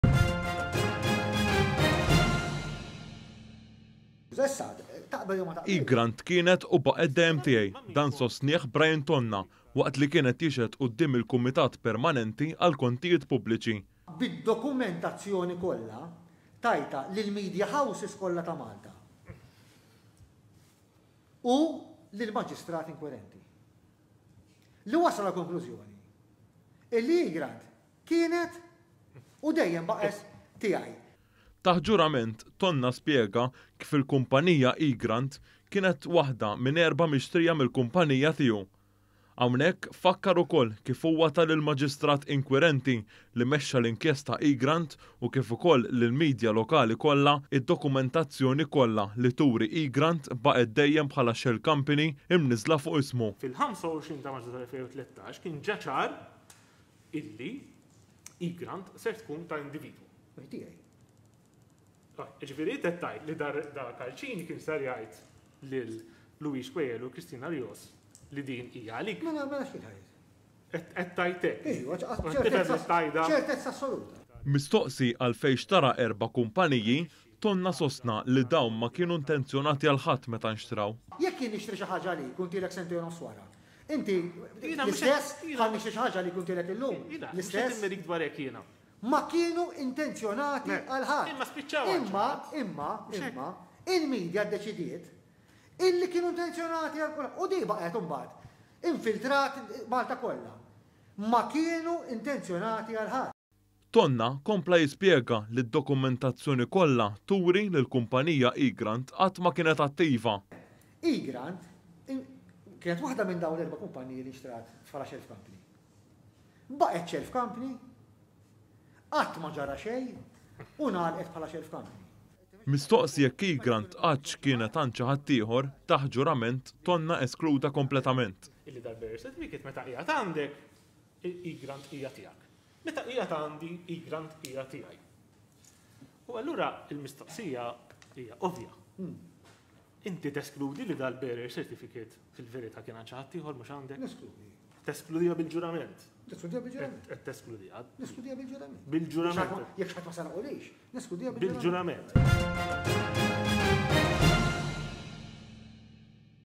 “EGRANT KIENET U DEJJEM BAQGĦET TIEGĦI” – BRIAN TONNA
ONE.COM.MT • Waqt seduta tal-Kumitat Parlamentari għall-Kontijiet Pubbliċi, Brian Tonna kkonferma taħt ġurament li Egrant dejjem baqgħet tiegħu u ma inxtrat minn ħadd.

Tonna baqa’ jispjega li l-erbgħa kumpaniji fil-Panama ma nxtraw għal ħadd u nbiegħu sentejn u nofs wara li kienu nxtraw, wara li ħarġu dettalji personali ta’ klijent minn trust f’bank lokali.

KITBA: KYLE MIFSUD
FILMAT: PARLAMENT / ARKIVJI

Egrant kienet u dejjem baqgħet tiegħi.

Dan sostnih Brian Tonna waqt li kien qed jixhed quddiem il-Kumitat Permanenti għall-Kontijiet Pubbliċi.

BRIAN TONNA
“Id-dokumentazzjoni kollha, tajtha lil media houses kollha u lill-Maġistrat Inkwirenti li wasal għall-konklużjoni li Egrant kienet u dejjem baqgħet tiegħi”

Taħt ġurament, Tonna spjega kif il-kumpanija Egrant kienet waħda minn erbgħa mixtrija mill-kumpanija tiegħu. Hawnhekk fakkar ukoll kif huwa ta lill-Maġistrat inkwirenti li mexxa l-inkjesta Egrant u kif ukoll lil midja kollha lokali d-dokumentazzjoni kollha li turi li Egrant baqgħet dejjem bħala shell company mniżżla fuq ismu.

“Karol Aquilina: “Fil-25 ta’ Marzu 2013 kien ċar li Egrant ħa tkun ta’ individwu.”
Brian Tonna: “Tiegħi”
Karol Aquilina: “Int qed tgħid li Karl Cini kien se jgħid li din kien għalik?”
Brian Tonna: “Iva mela, b’ċertezza assoluta”

Mistoqsi għalfejn xtara erba’ kumpaniji, Tonna sostna li dawn ma kienu intenzjonati għal ħadd meta nxtraw.

“Jekk jien nixtri xi ħaġa għalik u ntihielek sentejn u nofs wara inti l-istess jekk nixtri xi ħaġa għalik u ntihielek illum? Ma kienu intenzjonati għal ħadd imma il-media ddeċidiet li kienu intenzjonati u din infiltrat kullimkien. Ma kienu intenzjonati għal ħadd”

Tonna kompla jispjega li d-dokumentazzjoni kollha turi li l-kumpanija Egrant qatt ma kienet attiva.

“Egrant kienet waħda minn dawn l-erbgħa li nxtrat bħala shelf company, baqgħet shelf company, qatt ma ġara xejn u baqgħet bħala shelf company”

Mistoqsi jekk Egrant qattx kien għand xi ħadd ieħor, taħt ġurament Tonna eskluda kompletament.

“Karol Aquilina: “Dan iċ-ċertifikat meta qieħed għandek Egrant hija tiegħek, meta qiegħed għandi Egrant hija tiegħi. Inti teskludi li dan kien għand xi ħadd ieħor mhux għandek?”
Brian Tonna: “Neskludiha bil-ġurament. Jekk xi ħadd ma seraquliex, neskludiha bil-ġurament”